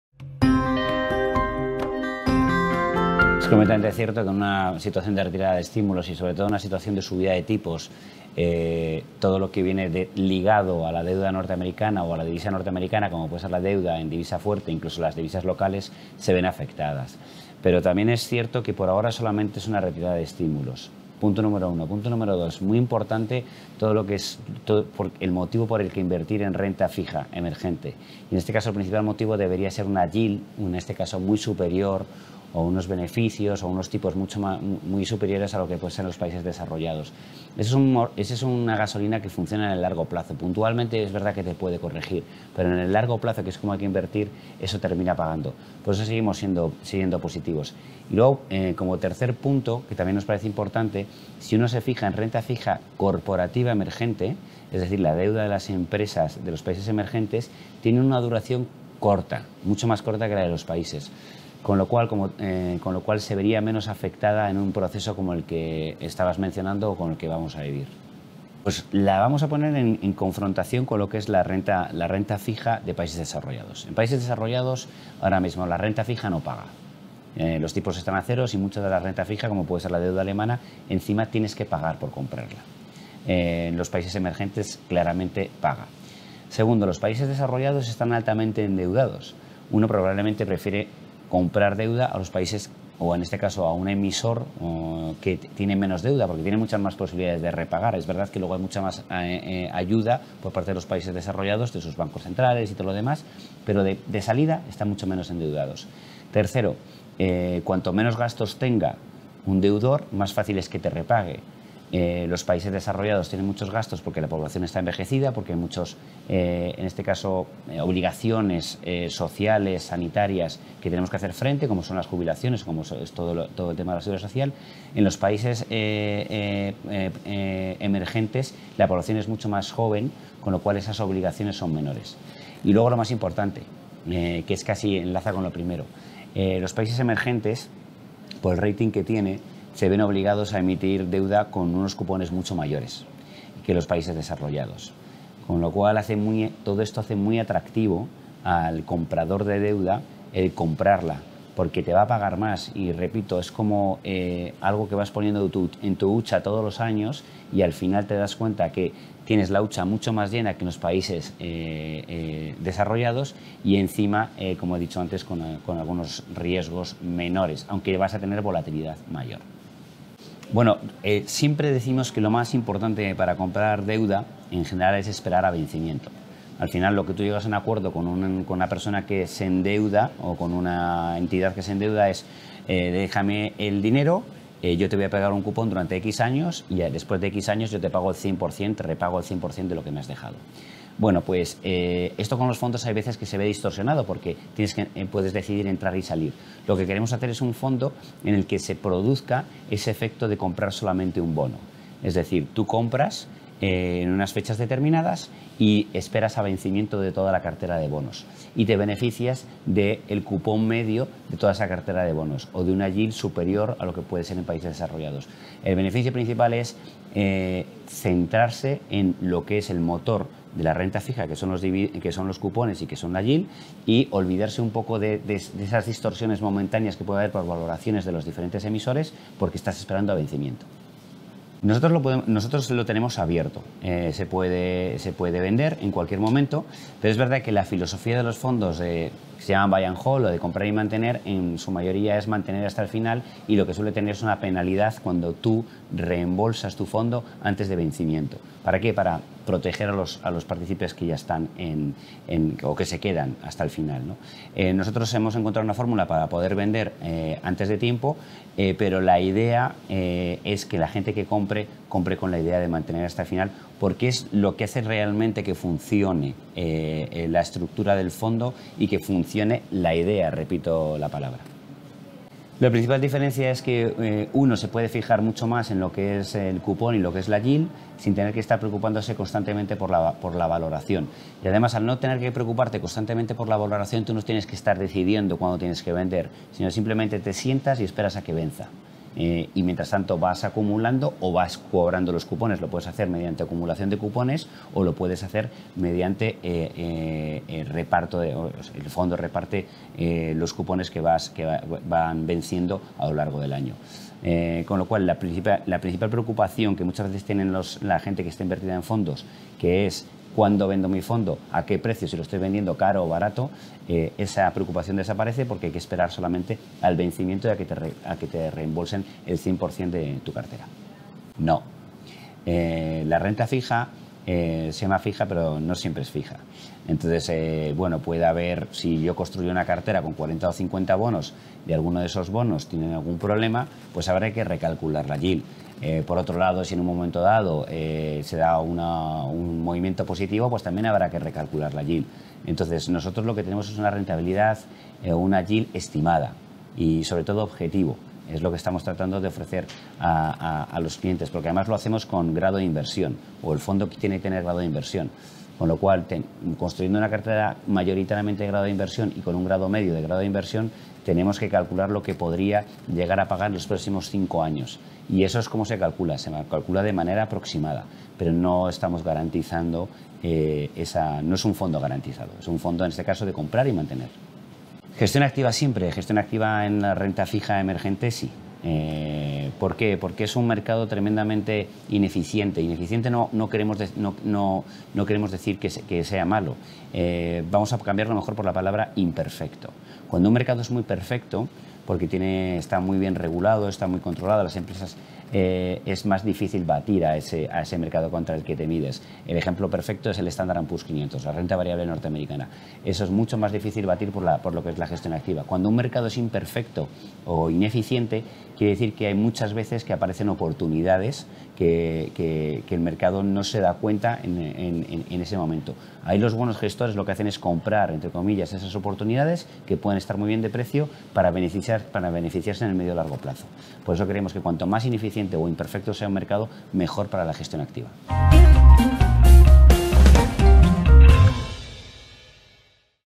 La RENTA FIJA CORPORATIVA EMERGENTE tienen una DURACIÓN CORTA
Demografía más fuerte y menos envejecida y un menor endeudamiento, son algunas de las características de los países emergentes. Si miramos a su renta fija, la volatilidad es una cualidad a tener en cuenta a la hora de invertir, pero también lo son unos cupones mucho mayores y, si nos centramos en la renta fija corporativa, su duración corta, tal y como explica Ricardo Comín, director de ventas para Iberia de Vontobel AM.

📌 Apúntate gratuitamente a nuestro boletín de Estrategias de Inversión y recibe análisis y recomendaciones para invertir